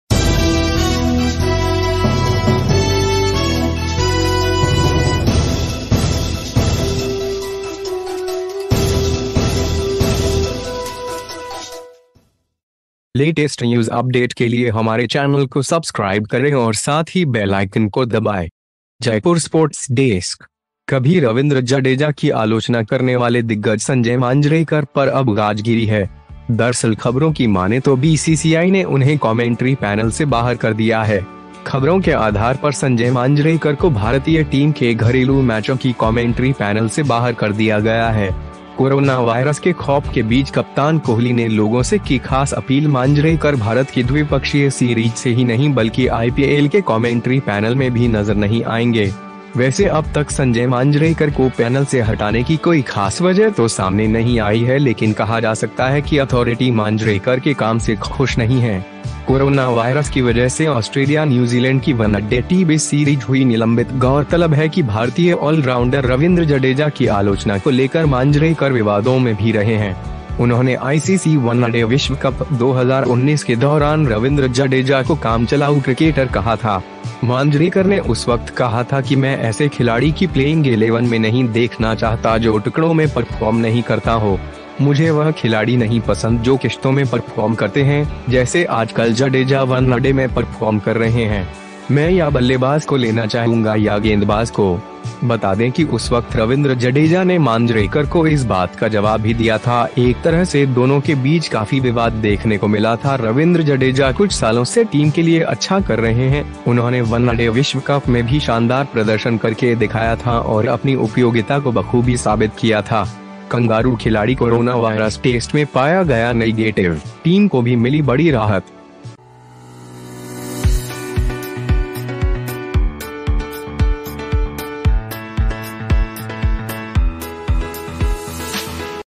गैलरी रविंद्र जडेजा पर सवाल उठाने वाले इस दिग्गज की अब बीसीसीआई ने की छुट्टी
The veteran who questioned the gallery Ravindra Jadeja has now been discharged by the BCCI at the Jaipur Sports Desk. The veteran Sanjay Manjrekar, who once criticized Ravindra Jadeja, has now fallen furious. In fact, if the news is to be believed, the BCCI has excluded him from the commentary panel. Based on the news, Sanjay Manjrekar has been dropped from the commentary panel of the Indian team home matches. Amid the fear of Corona virus, Captain Kohli appealed to the people, Manjrekar will not be seen in the IPL commentary panel, not only from India bilateral series. By the way, no specific reason has been revealed so far to remove Sanjay Manjrekar from the panel but it can be said that the authorities are not happy with Manjrekar work. Suspended of Australia-New Zealand ODI-T20 series due to Corona virus, it is noteworthy that Manjrekar has also been in controversies regarding the criticism of Indian all-rounder Ravindra Jadeja. He called Ravindra Jadeja a part-time cricketer during the ICC ODI World Cup 2019. Manjrekar said at the time that I do not want to see a playing XI of a player who does not perform in pieces. I dont like players who perform in installments, like Jadeja is performing in ODIs nowadays. I would like to take either the batsman or the bowler. Let me tell you that at that time Ravindra Jadeja had also responded to Manjrekar, in a way there was a lot of controversy between the two. Ravindra Jadeja has been doing well for the team for a few years, he has also done well in the ODI World Cup. Had demonstrated and proved its usefulness. Kangaroo player found negative in corona virus test, team also got big relief

VR NEWS !!